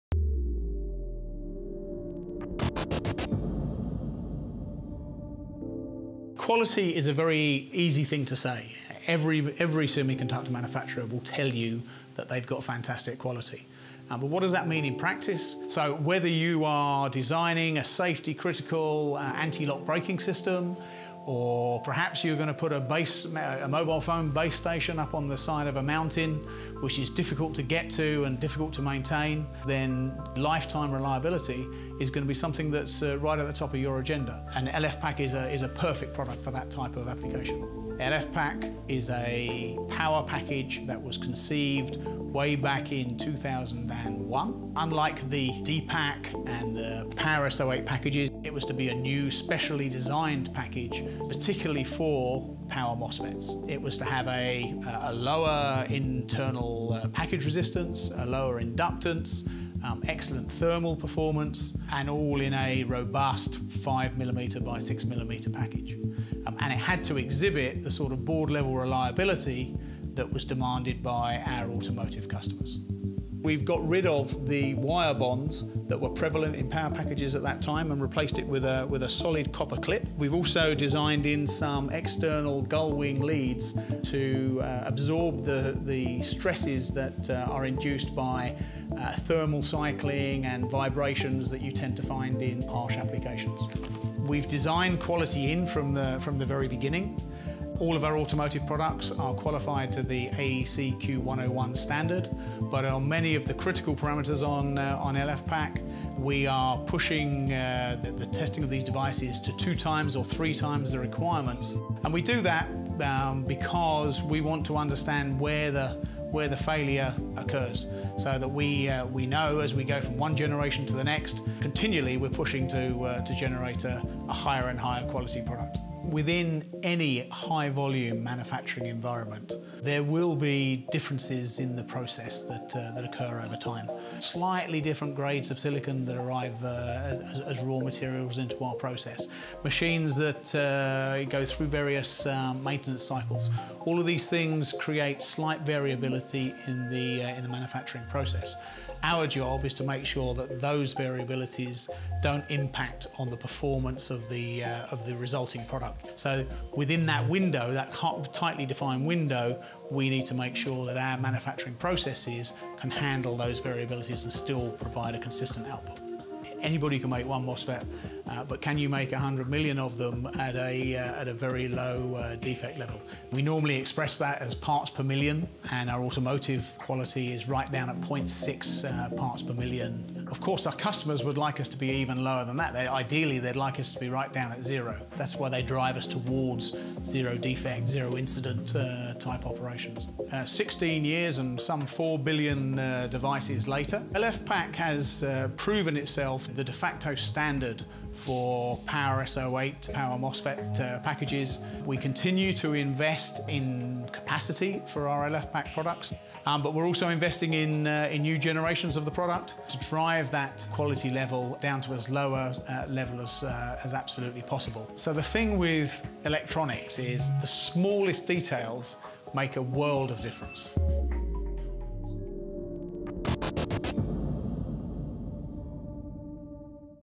Learn how the LFPAK became the de facto power packaging standard
Every semiconductor manufacturer will tell you they’ve got fantastic quality. But what does that mean in practice. Our MOSFET Marketing & Product Group Director, Chris Boyce, explains how quality and lifetime reliability has been at the heart of our LFPAK development.